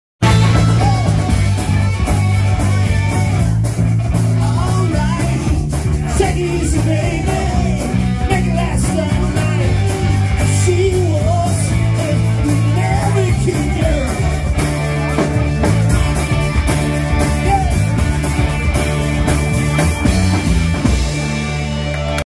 2023 07 30 01 23 32 Dana singing with the Ragdoll classic rock band at The Fling Cocktail Lounge
Dana singing with the Ragdoll classic rock band at The Fling Cocktail Lounge, Santa Ana, California,